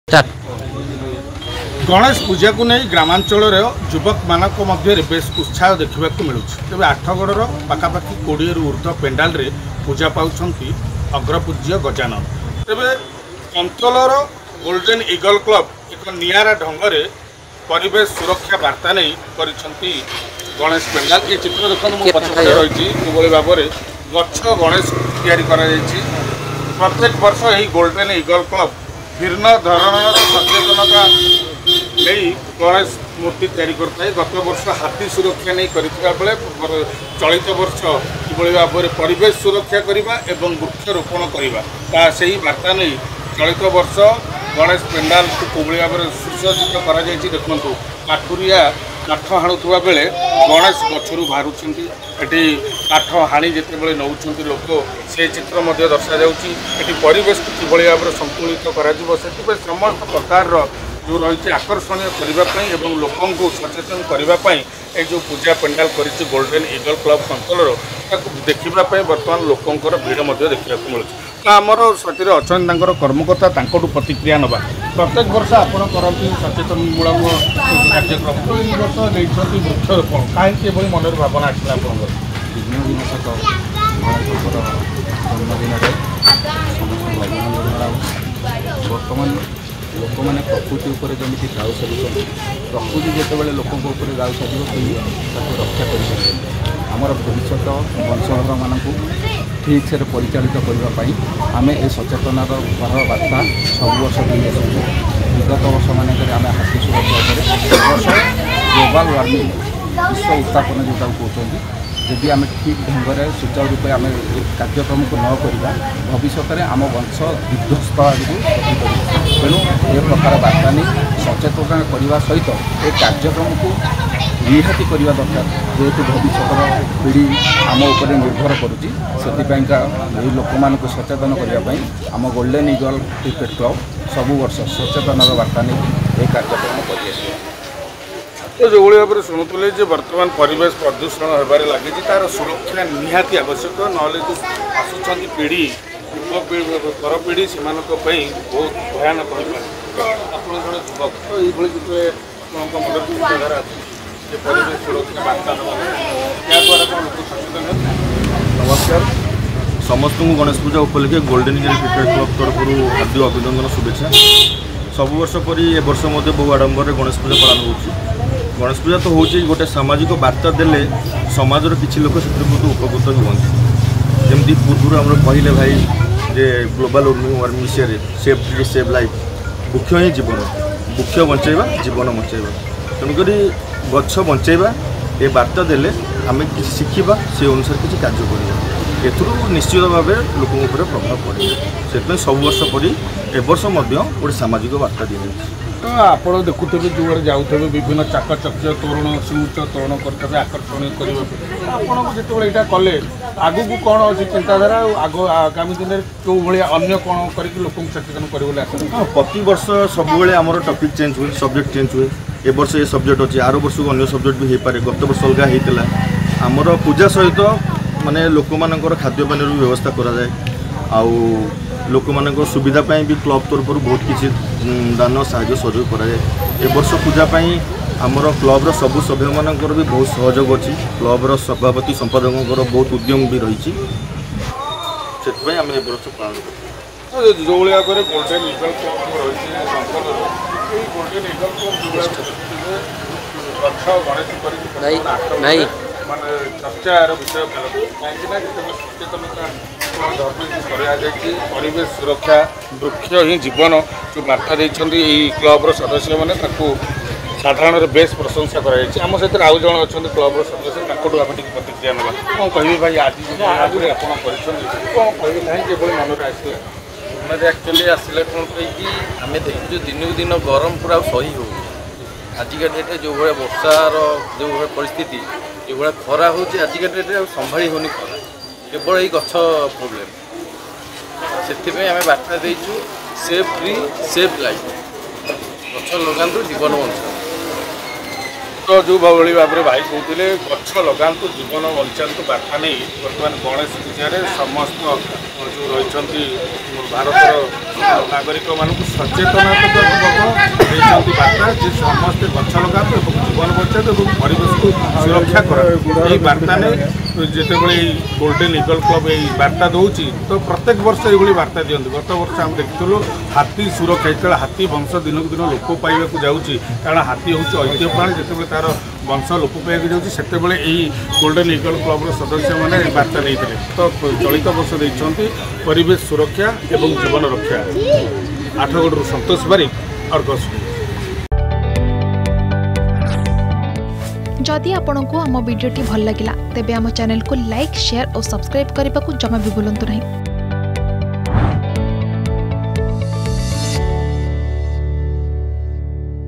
ଆଠଗଡ଼ରେ ଗଛ ଗଣେଶ || Ganesh Puja || Athagarh
ଆଠଗଡ଼ର କନ୍ତୋଲ ଗୋଲଡେନ ଇଗଲ କ୍ଲବ ତରଫରୁ ଗଛ ଲଗାନ୍ତୁ ଜୀବନ ବଞ୍ଚାନ୍ତୁ ସଚେତନତା ବାର୍ତ୍ତା ନେଇ ଗଛ ଗଣେଶ ମୂର୍ତ୍ତି ତିଆରି କରି ଆଠଗଡ଼ ଅଞ୍ଚଳରେ ବେଶ ଚର୍ଚ୍ଚାର ବିଷୟ ପାଲଟିଛନ୍ତି।